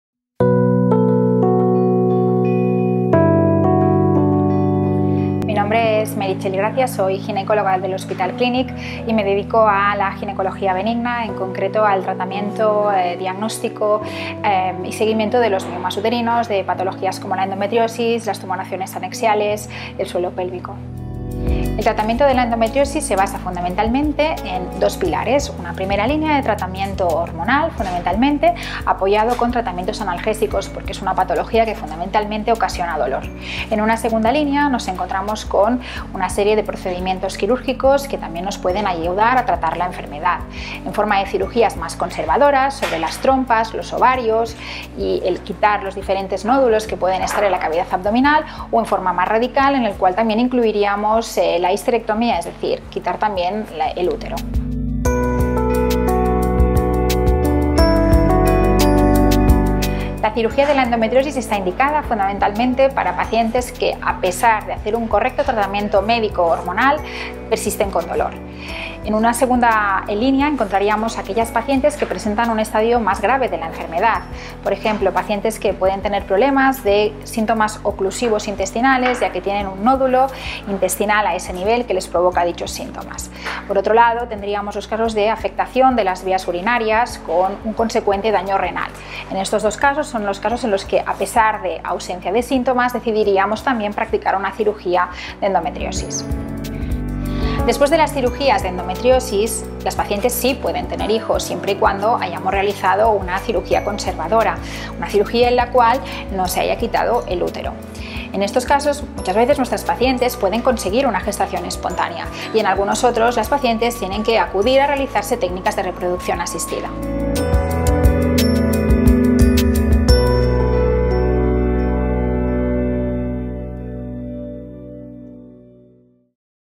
ENDOMETRIOSIS Y EMBARAZO ¿Es posible? Tratamiento de la endometriosis y cuándo se recomienda cirugía
La Dra. Meritxell Gracias explica en este vídeo el tratamiento de la endometriosis, cuándo está indicada la cirugía de la endometriosis y sus consecuencias en la fertilidad.
El tratamiento se basa en dos pilares: Una primera línea de tratamiento hormonal, apoyado con analgésicos; y una segunda línea basada en la cirugía, que puede ayudar a tratar la enfermedad...

👩‍⚕️ MÁS INFORMACIÓN SOBRE LA DRA. MERITXELL GRACIA